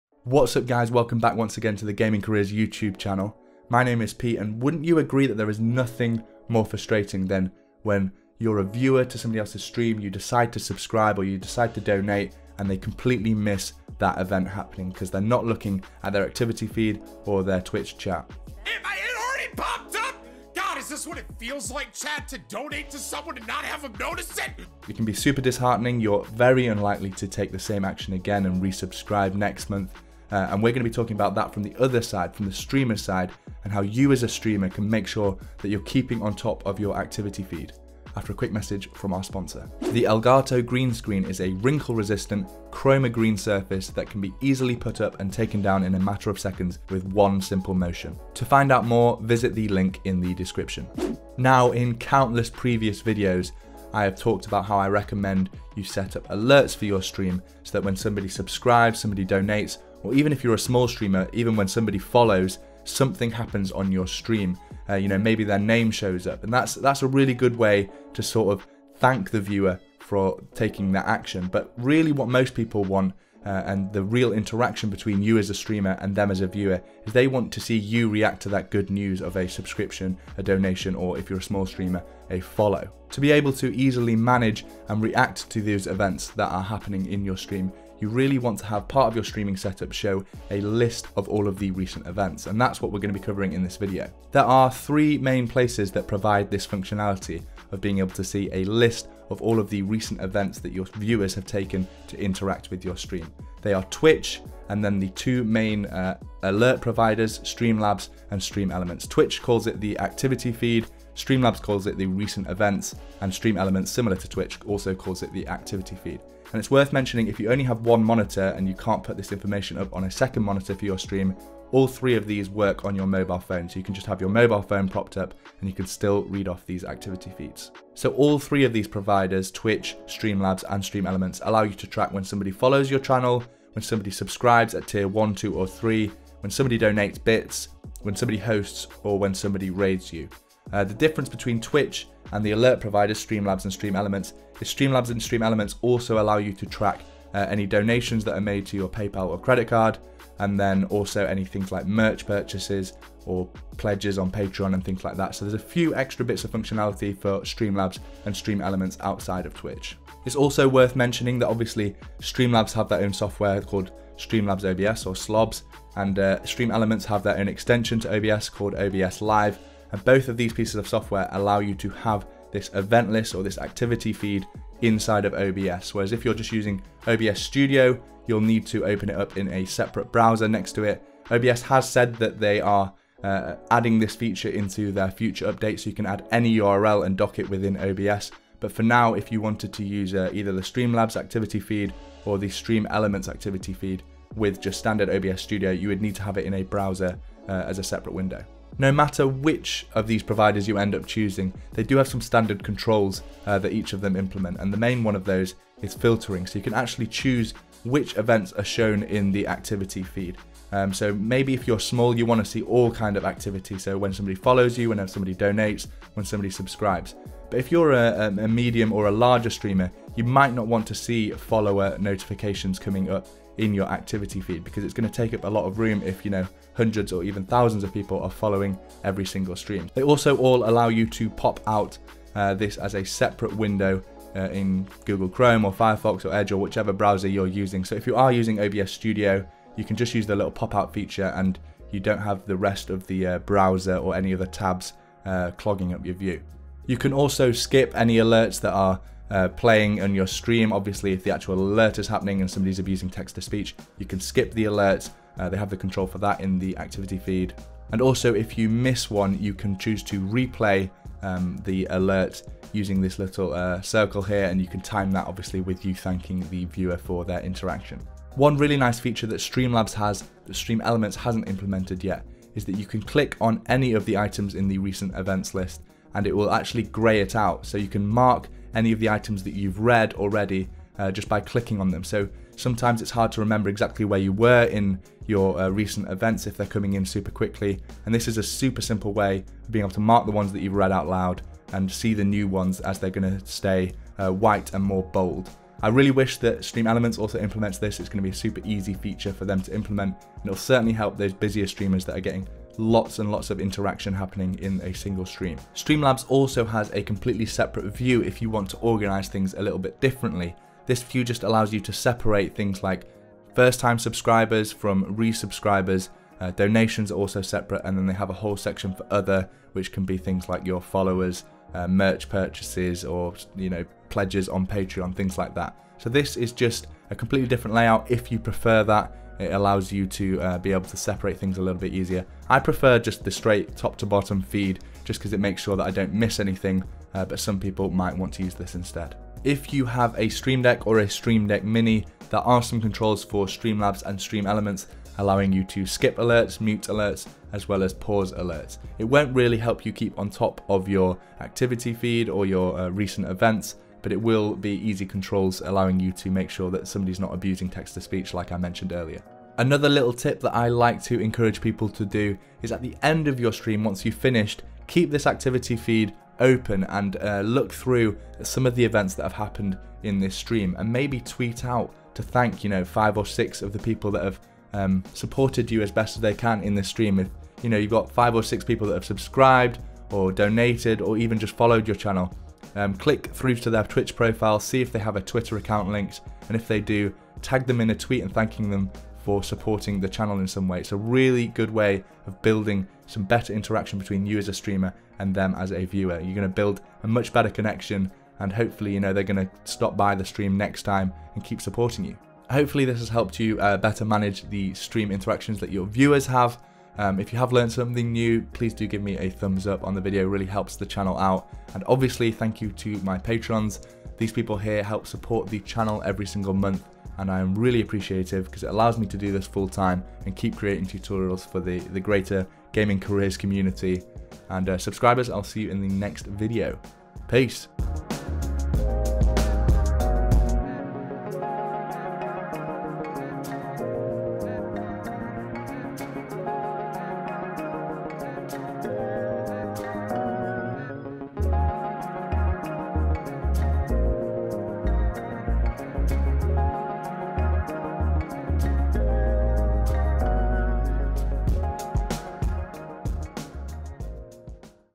Never Miss a Subscription, Donation or Host by MASTERING the Activity Feed!
There's nothing more frustrating for a viewer than you missing their Sub or Donation. In this video, we look at Twitch, Streamlabs, and StreamElements Activity Feeds so you'll never miss an alert for your stream ever again!

📌 Resources mentioned:
Elgato Green Screen

⏩ Timestamps:
0:00 Introduction
1:33 Where to see your Activity Feeds?
2:08 Comparing Twitch, Streamlabs and StreamElements
2:40 Streamlabs OBS, OBS.Live and OBS Studio
3:18 Filtering Out Activity in the Feed
4:08 Skipping and Replaying Alerts
4:29 Marking an Event as Read
5:03 Streamlabs Activity View
5:38 Stream Deck Controls
5:59 Thanking Your Viewers

(My full setup, mics, cameras, and software recommendations)

(Weekly streaming news, tips, and guides)

(Written guides and free streaming tools)